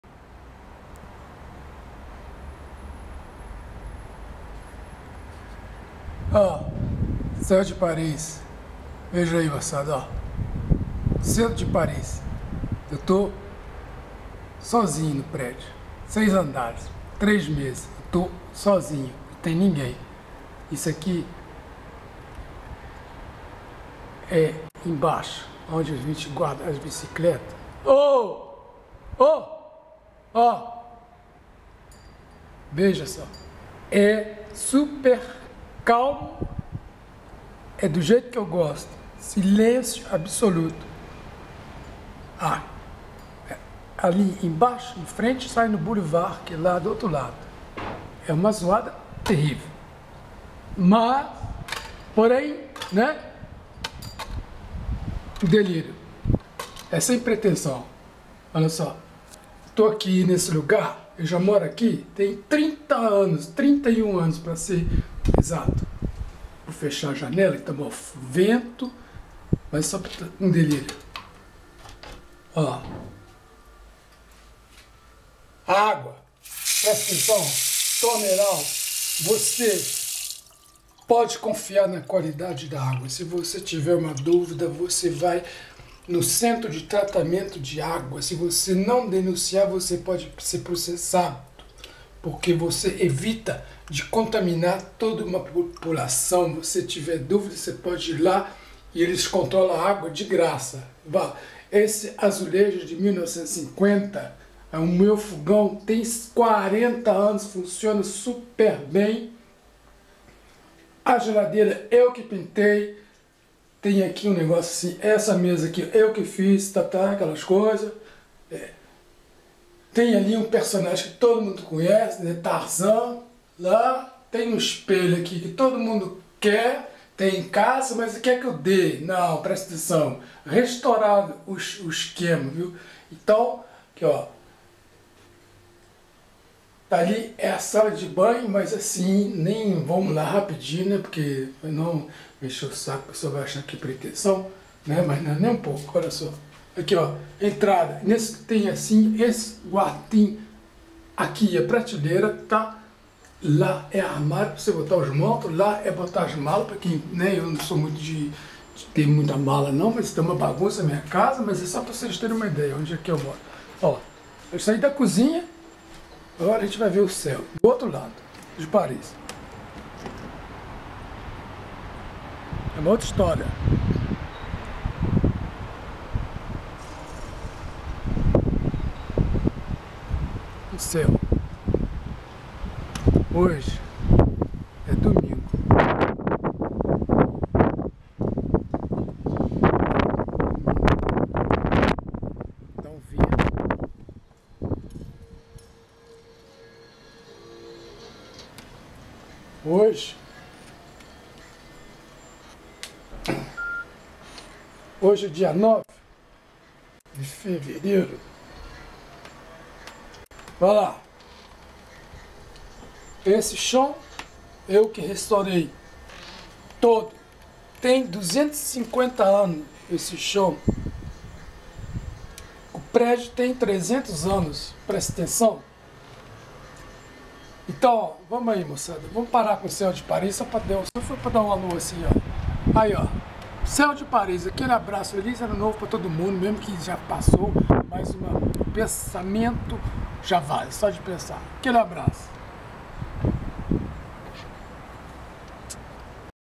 Chez moi!
Aujourd'hui dimanche 9 fév.2014, 7 degrés j'ai fait ce cour filme car je suis seul dans le bâtiment un calme absout on voie le ciel azur dans la cour et l'outre vue da la rue Jean Beau sire a Paris ma rue  je suis depuis 31 an  et heureux d'être dans la ville lumière!